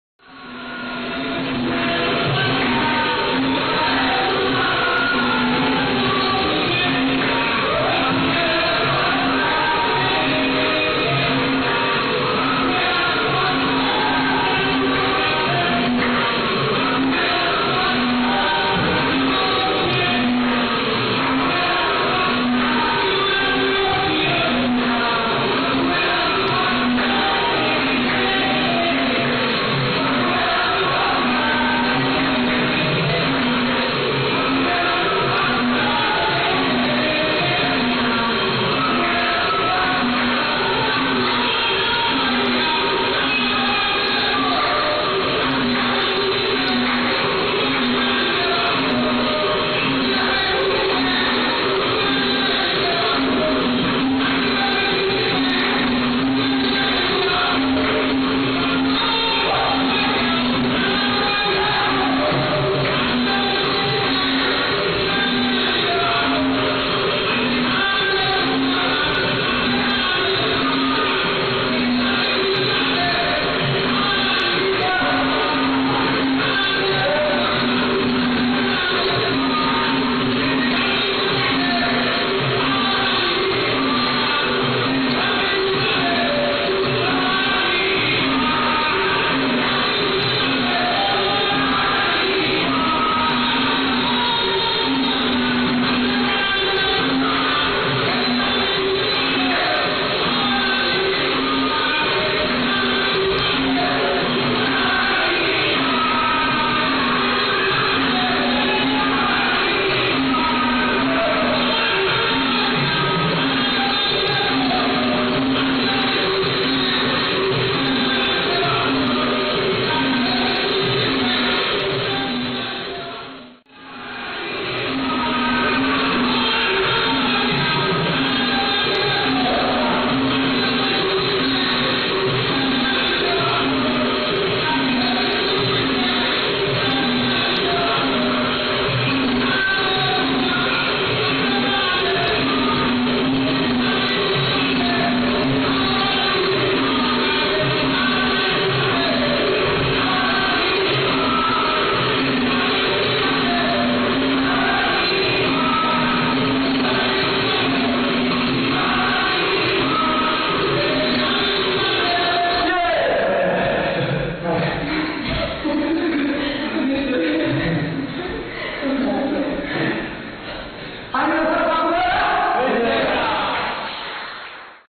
Roda Aula de Capoeira pegando com os Alunos Japan dia 05/10/2011
Capoeira Corrente Negra Contra Mestre Renato Leão